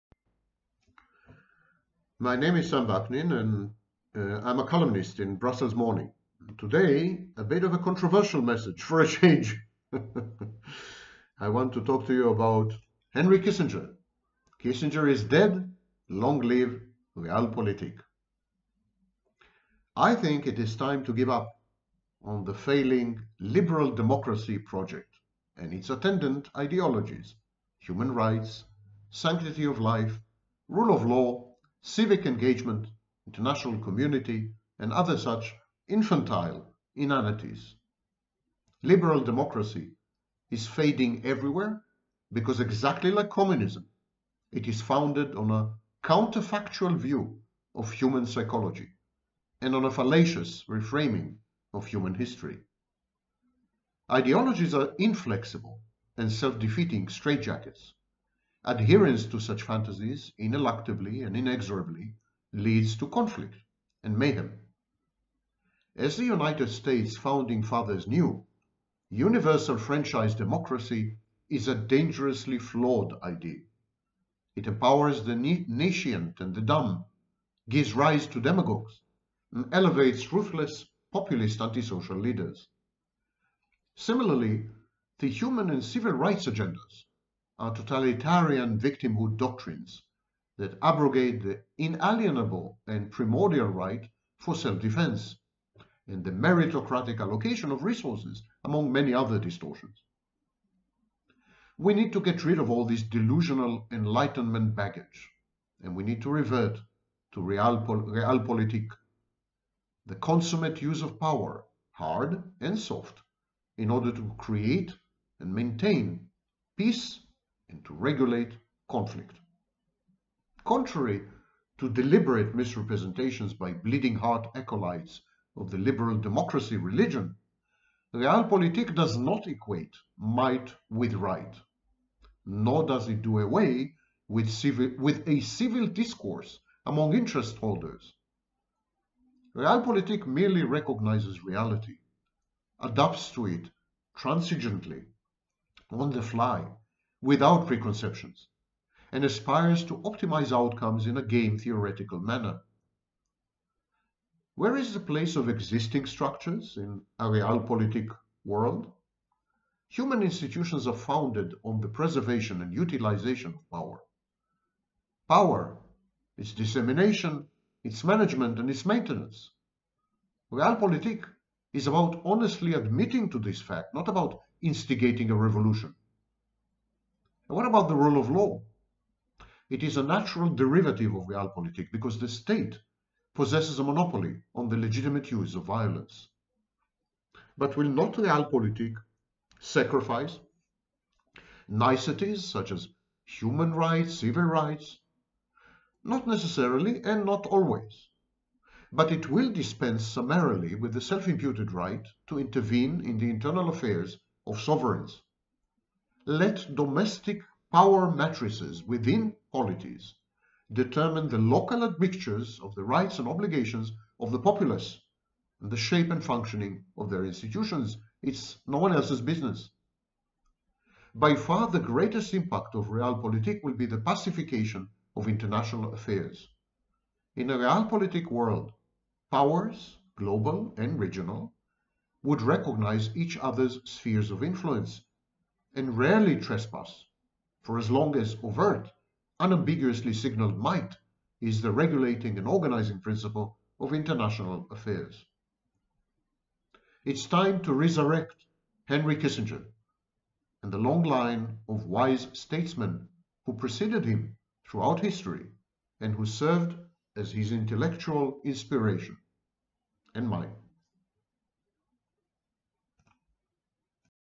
Kissinger is Dead, Long Live Realpolitik (Brussels Morning)
Column here:

It is time to give up on the failing liberal democracy project and its attendant ideologies: human rights, sanctity of life, rule of law, civic engagement, international community, and other such infantile inanities.

Liberal democracy is fading everywhere because exactly like Communism it is founded on a counterfactual view of human psychology and on a fallacious reframing of human history.

Ideologies are inflexible and self-defeating straightjackets. Adherence to such fantasies ineluctably and inexorably leads to conflict and mayhem.

As the USA’s Founding Fathers knew, universal franchise democracy is a dangerously flawed idea. It empowers the nescient and the dumb, gives rise to demagogues, and elevates ruthless, populist antisocial leaders.

Similarly the human and civil rights agendas are totalitarian victimhood doctrines that abrogate the inalienable and primordial right for self-defense and the meritocratic allocation of resources, among many other consequent distortions.

We need to get rid of all this delusional enlightenment baggage and revert to Realpolitik: the consummate use of power, hard and soft, to create and maintain peace and to regulate conflict.

Contrary to deliberate misrepresentations by bleeding heart acolytes of the liberal democracy religion, Realpolitik does not equate might with right nor does it do away with a civil discourse among interest-holders. It merely recognizes reality, adapts to it transigently, on the fly, without preconceptions, and aspires to optimize outcomes in a game theoretical manner.

Where is the place of existing structures in a Realpolitik world?
Human institutions are founded on the preservation of utilization of power: its dissemination, its management, and its maintenance. Realpolitik is about honestly admitting to this fact, not about instigating a revolution.

What about the rule of law? It is a natural derivative of Realpolitik because the state possesses a monopoly on the legitimate use of violence.

But will not Realpolitik sacrifice niceties such as human and civil rights? Not necessarily and not always. But it will dispense summarily with the self-imputed right to intervene in the internal affairs of sovereigns. Let domestic power matrices within polities determines the local admixtures of the rights and obligations of the populace and the shape and functioning of their institutions.

By far the greatest impact of Realpolitik will be the pacification of international affairs. In a Realpolitik world, powers – global and regional – would recognize each other’s spheres of influence and rarely trespass for as long as overt, unambiguously signalled might is the regulating and organizing principle of international affairs.

Time to resurrect Henry Kissinger and the long line of wise statesmen who preceded him throughout history and who served as his intellectual inspiration.